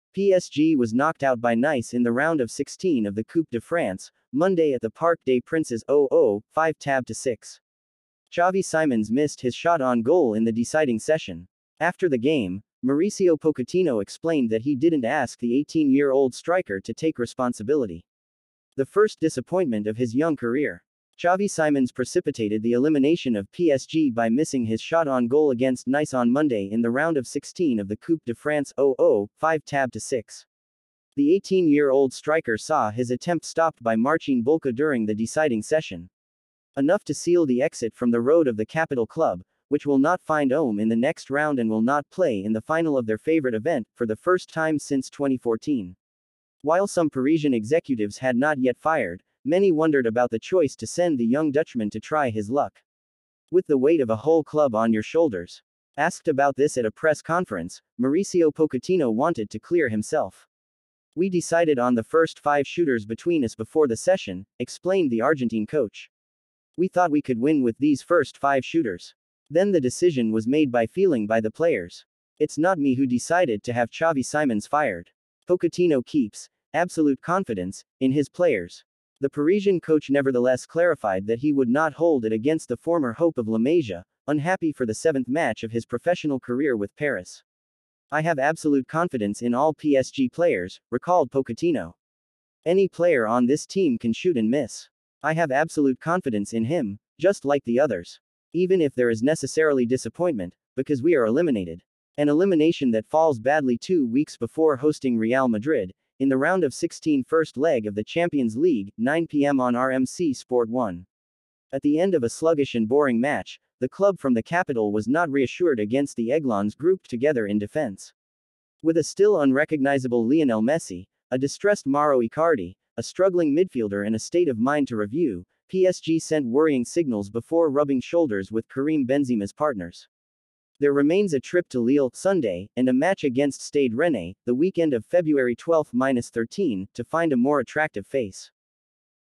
‘It wasn’t my decision’, Pochettino clears up after Xavi Simons shot on goal
PSG was knocked out by Nice in the round of 16 of the Coupe de France, Monday at the Parc des Princes (0-0 5 tab to 6). Xavi Simons missed his shot on goal in the deciding session. After the game, Mauricio Pochettino explained that he didn’t ask the 18-year-old striker to take responsibility.